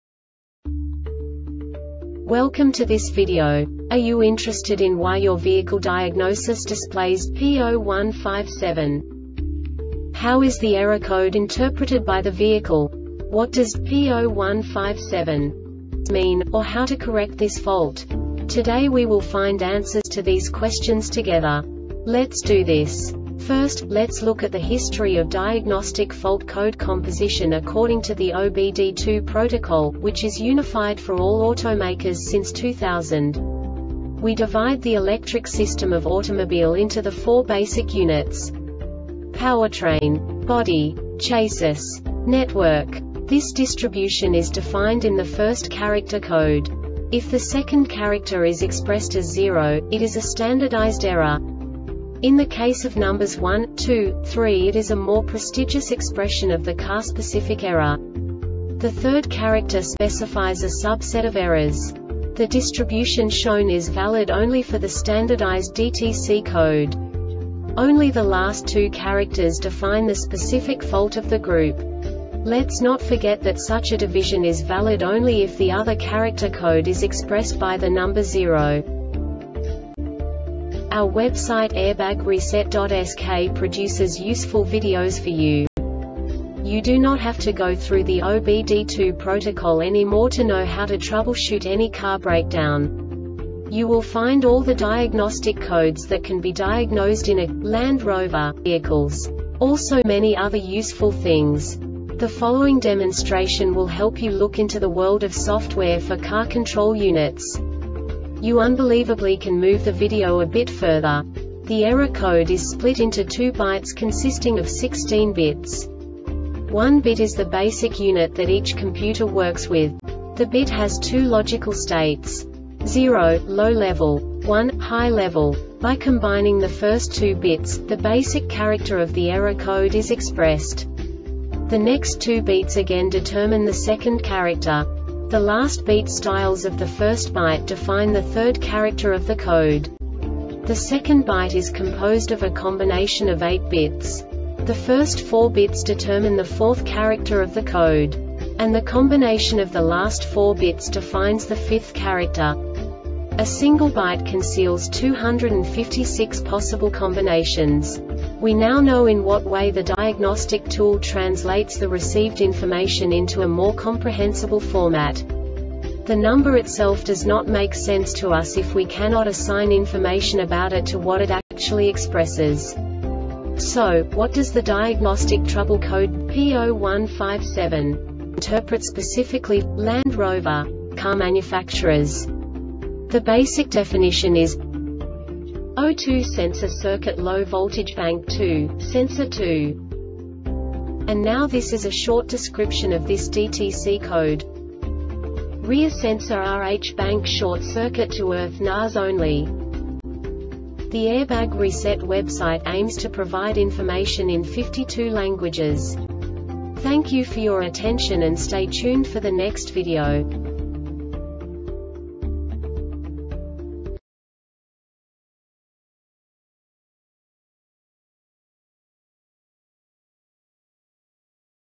DTC Landrover P0157 Short Explanation
Contents:
0:21 Basic DTC analysis according to OBD2 protocol standard.
1:48 Insight into programming
2:58 A diagnostically understandable description of the Diagnostic Trouble Code
3:05 Basic error definition

Make
Land Rover
Code
P0157
Definition
O2 sensor circuit low voltage bank 2, sensor 2
Description
Rear sensor RH bank short circuit to earth NAS only